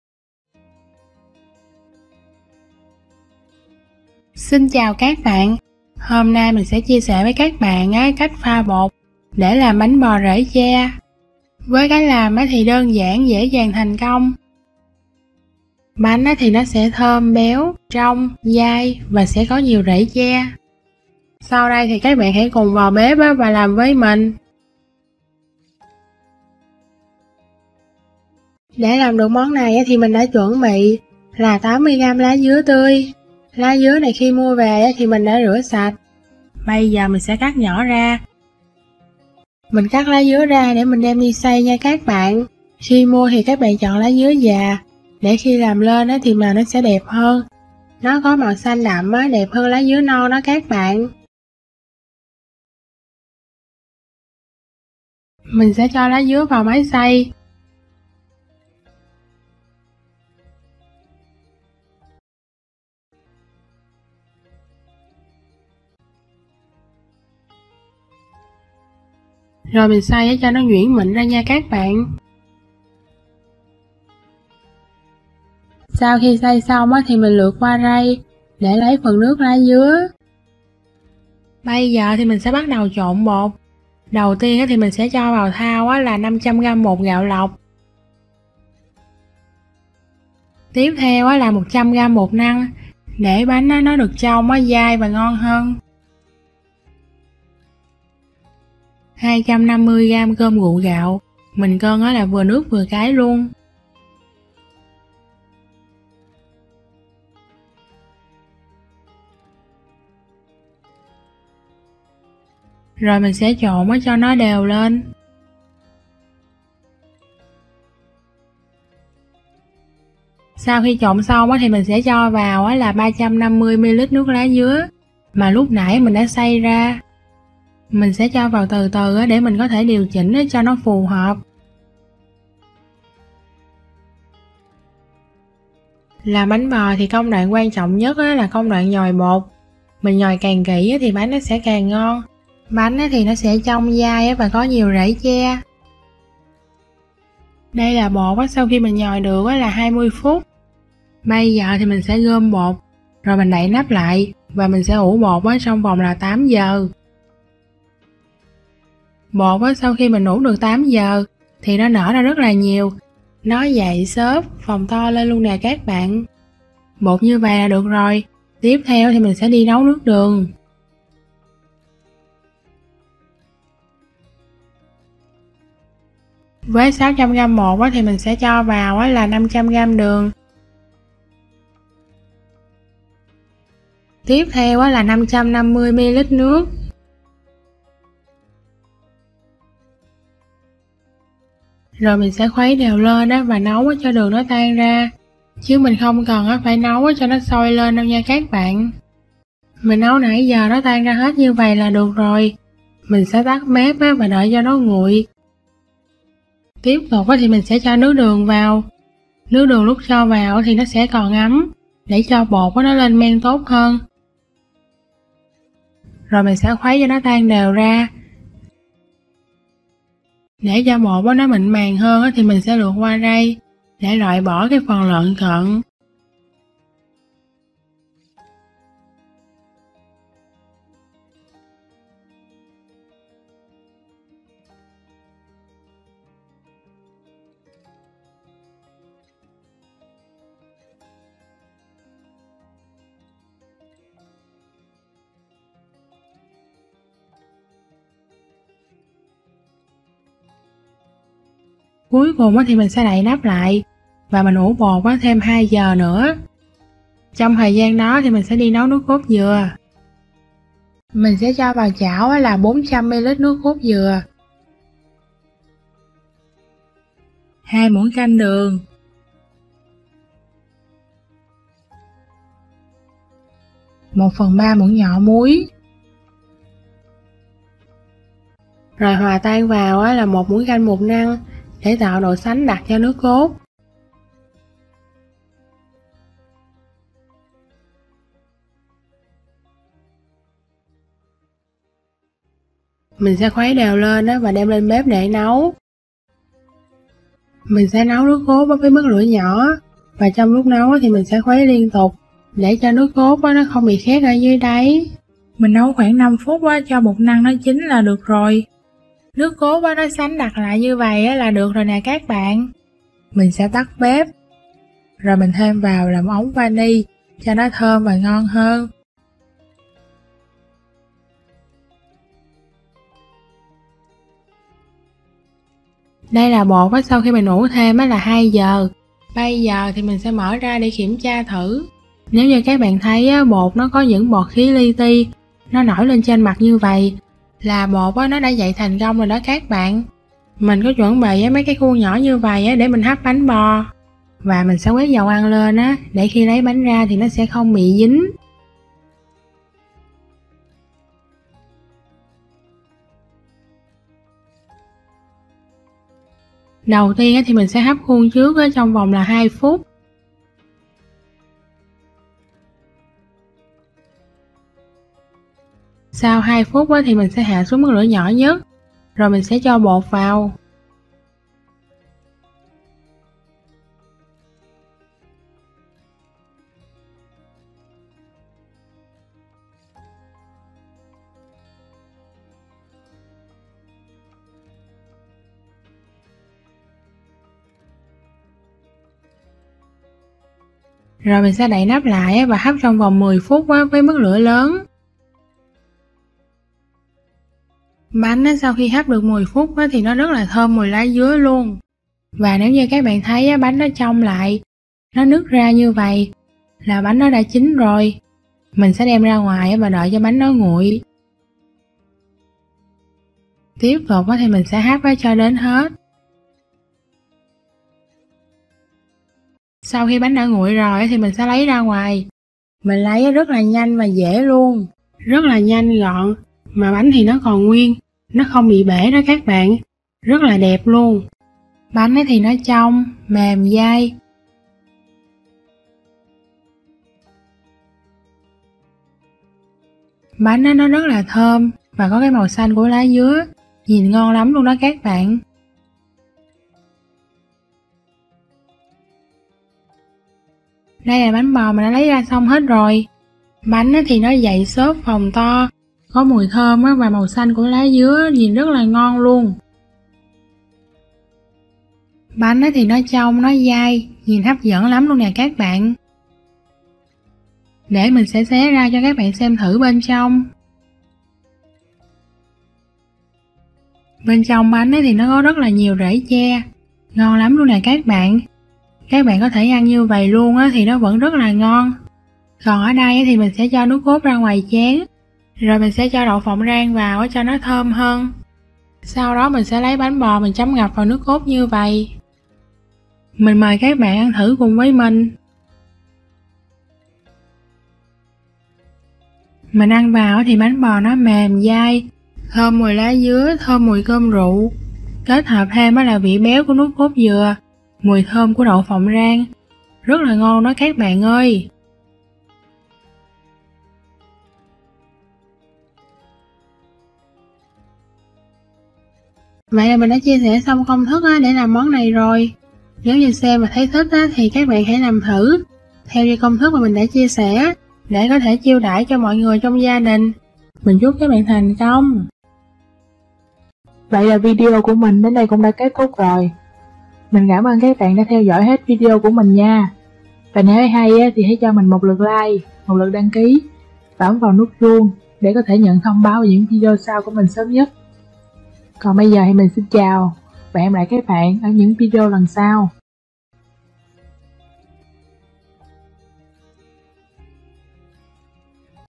Cách pha bột làm BÁNH BÒ RỄ TRE đơn giản dễ thành công bánh trong dai thơm béo ngon tuyệt vời
Cách pha bột làm BÁNH BÒ RỄ TRE đơn giản dễ thành công bánh trong dai thơm béo ngon tuyệt vời . Các bạn cùng vào bếp làm với mình nhé!
Nguyên liệu
500g bột gạo lọc
100g bột năng
250g cơm rượu gạo
350ml nước lá dứa để nhồi bột (xay từ 80g lá dứa tươi )
500g đường
550ml nước
Phần nước cốt dừa
400ml nước cốt dừa
2 muỗng canh đường
1/3 muỗng cà phê muối
1 muỗng canh bột năng
1 ống bột vani